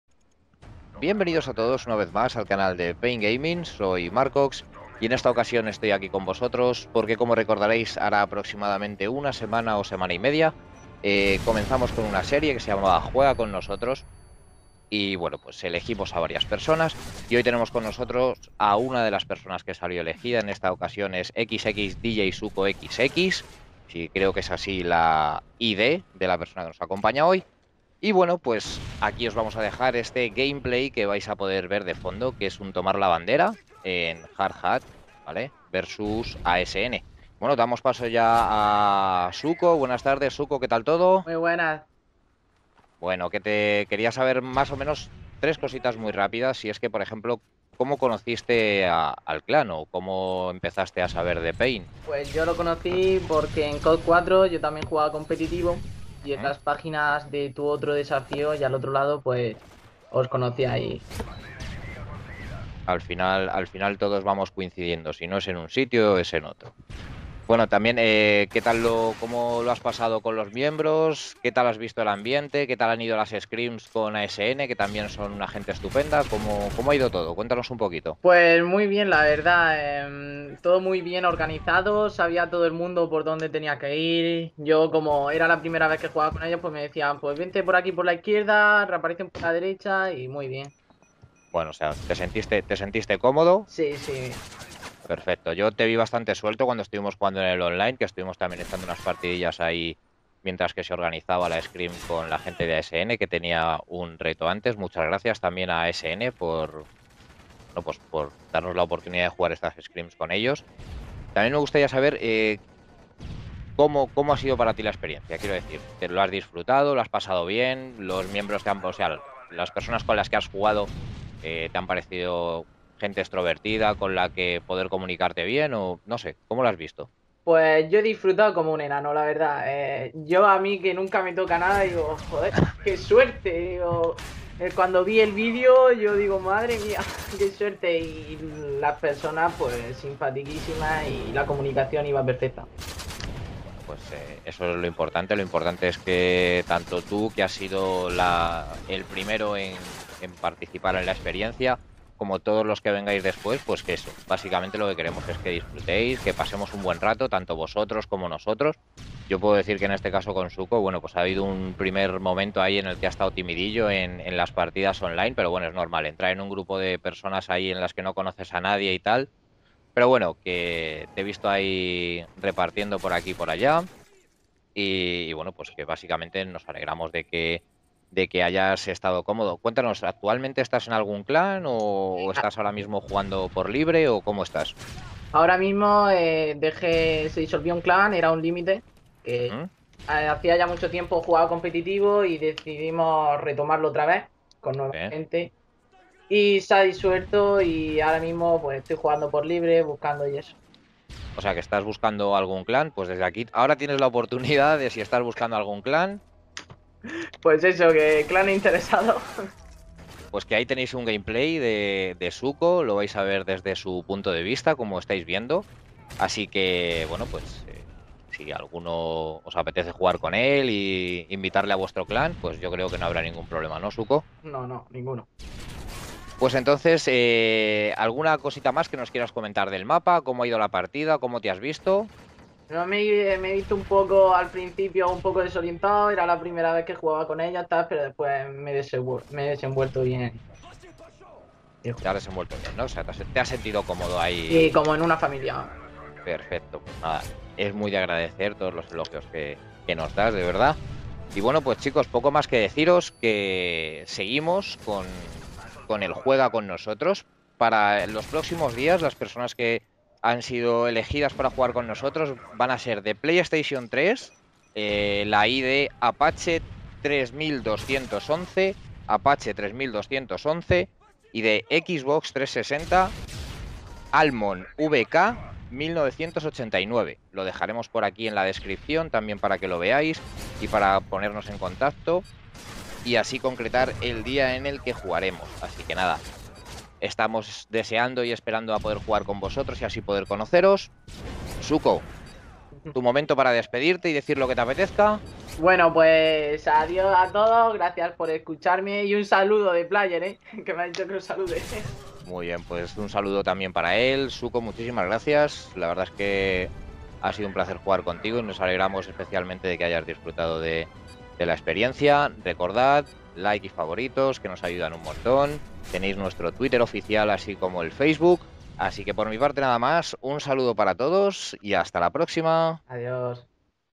¡Juega con nosotros! | Siguientes invitados y experiencia DjSuko
Like & Favorito nos ayuda a seguir!!

Aquí os dejamos la experiencia vivida por uno de los invitados la semana pasada, junto al gameplay con su cámara.

La ID de PS3 y GT de Xbox en jugar con nosotros en estos próximos días son las siguientes.

PS3: Apache3211  -  Xbox360: Almonvk1989

¡Dejar vuestros comentarios con IDs y GTs en este vídeo para las próximas invitaciones!